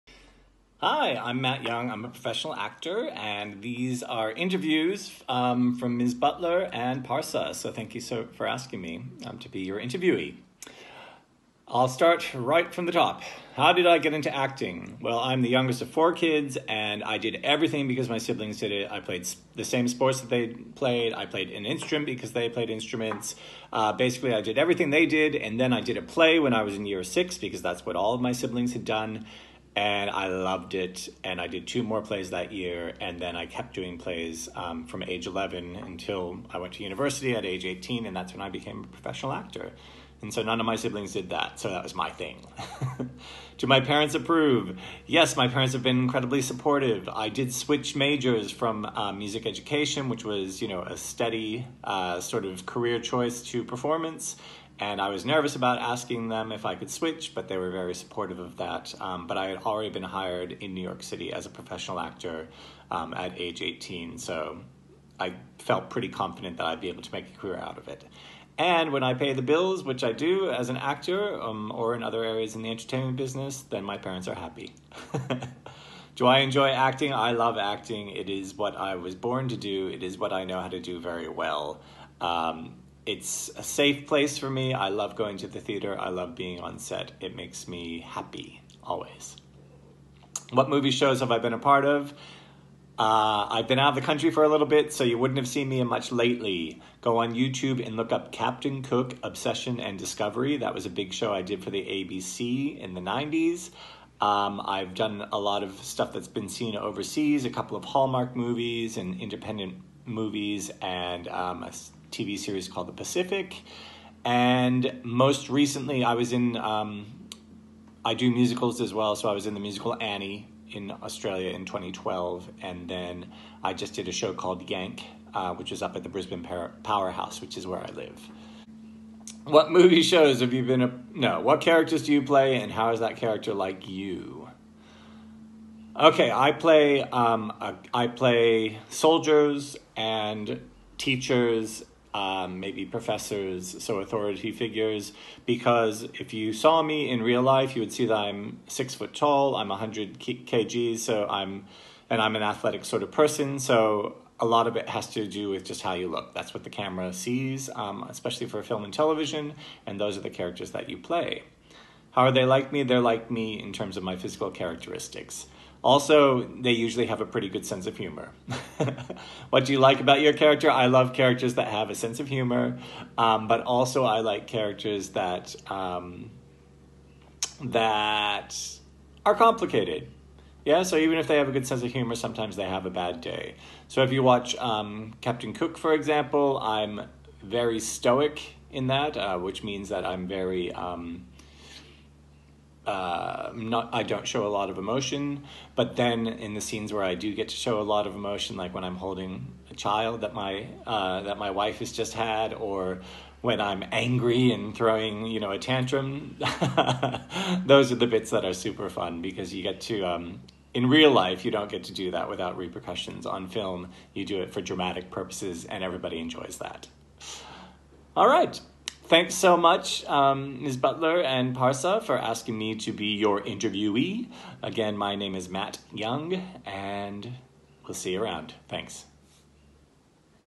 Matt Young Actor Interview for Primary School Student
During COVID in 2020, I did a lot of "pro bono" work for schools in  Australia and the USA, offering my services as a professional actor, acting coach, and a casting professional.

Here is an interview I did for a primary school student at that time.

It is relevant for actors of all ages and experiences.